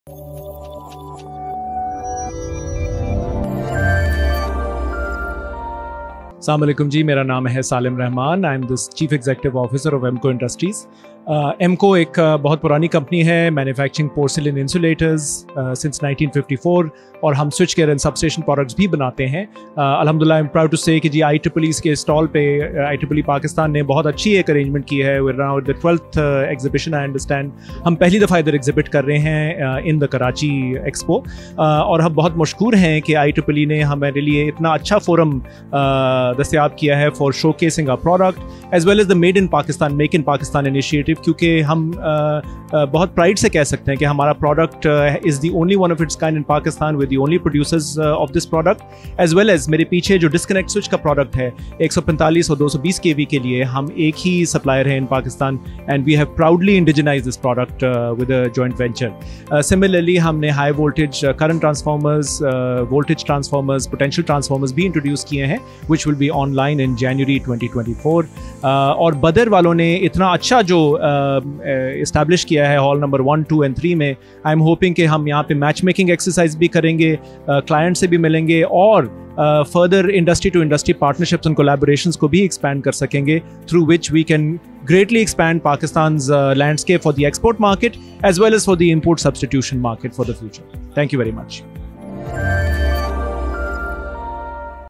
EMCO, our Proud Exhibitor, uncovering their innovative showcases at IEEEP Fair 2023.
IEEEP Fair 2023, organized by IEEE Pakistan and managed by Badar Expo Solutions, is the 12th edition of the premier exhibition showcasing cutting-edge innovations and industry advancements, bringing together leading technology companies and experts under one roof.

The exhibition is live at Expo Centre, offering a dynamic platform for networking and discovery.